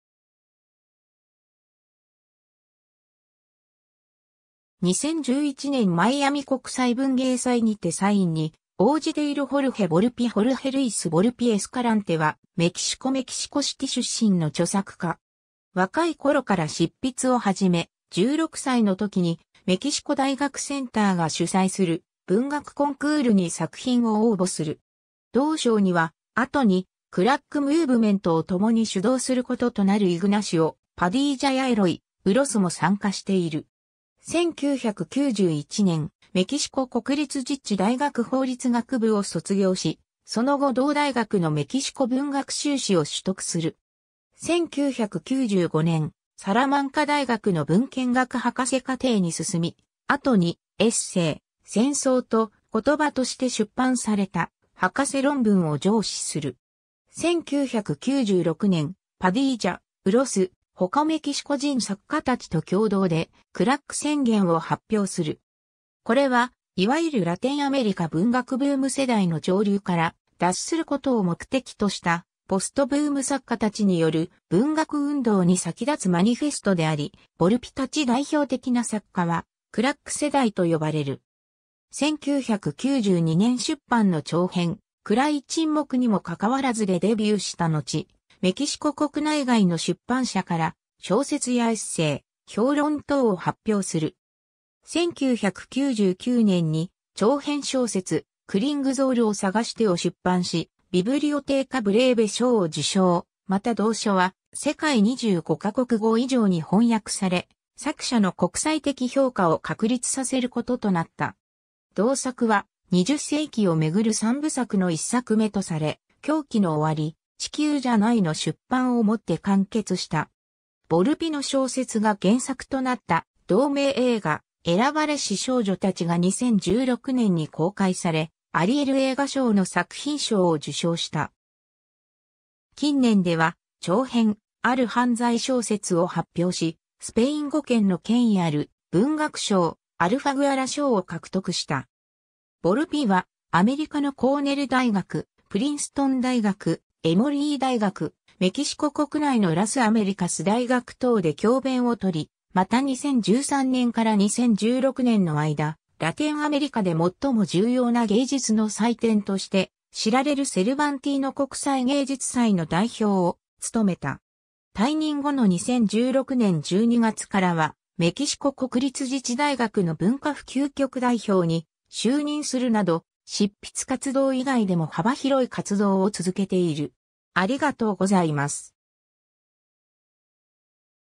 ホルヘ・ボルピ
2011年マイアミ国際文芸祭にてサインに応じているホルヘ・ボルピ ホルヘ・ルイス・ボルピ・エスカランテ（スペイン語: Jorge Luis Volpi Escalante, 1968年7月10日 - ）は、メキシコ・メキシコシティ出身の著作家。
 若い頃から執筆を始め、16歳のときにメキシコ大学センターが主宰する文学コンクールに作品を応募する。
同賞には、後に『クラック・ムーブメント』を共に主動することとなるイグナシオ・パディージャやエロイ・ウロスも参加している。
 1991年、メキシコ国立自治大学法律学部を卒業し、その後同大学のメキシコ文学修士を取得する。
1995年、サラマンカ大学の文献学博士課程に進み、後にエッセイ『戦争と言葉』として出版された博士論文を上梓する。
 1996年、パディージャ、ウロス、他メキシコ人作家たちと共同で『クラック宣言』を発表する。
これは、いわゆるラテンアメリカ文学ブーム世代の潮流から脱することを目的とした、ポストブーム作家たちによる文学運動に先立つマニフェストであり、ボルピたち代表的な作家は『クラック世代』と呼ばれる。
 1992年出版の長編『暗い沈黙にもかかわらず』でデビューしたのち、メキシコ国内外の出版社から小説やエッセイ、評論等を発表する。
1999年に長編小説『クリングゾールをさがして』を出版し、ビブリオテーカ・ブレーベ賞を受賞、また同書は世界25ヶ国語以上に翻訳され、作者の国際的評価を確立させることとなった。
同作は『20世紀をめぐる三部作』の1作目とされ、『狂気の終わり』(2003)、『地球じゃない』(2006)の出版をもって完結した。
ボルピの小説が原作となった同名映画『選ばれし少女たち』（ダビ・パブロ監督）が2016年に公開され、アリエル映画賞の作品賞を受賞した。
近年では、長編『ある犯罪小説』(2018)を発表し、スペイン語圏の権威ある文学賞アルファグアラ賞を獲得した。
 ボルピは、アメリカのコーネル大学、プリンストン大学、エモリー大学、メキシコ国内のラス・アメリカス大学等で教鞭をとり、また2013年から2016年の間、ラテンアメリカで最も重要な芸術の祭典として知られるセルバンティーノ国際芸術祭の代表を務めた。
退任後の2016年12月からはメキシコ国立自治大学の文化普及局代表に就任するなど執筆活動以外でも幅広い活動を続けている。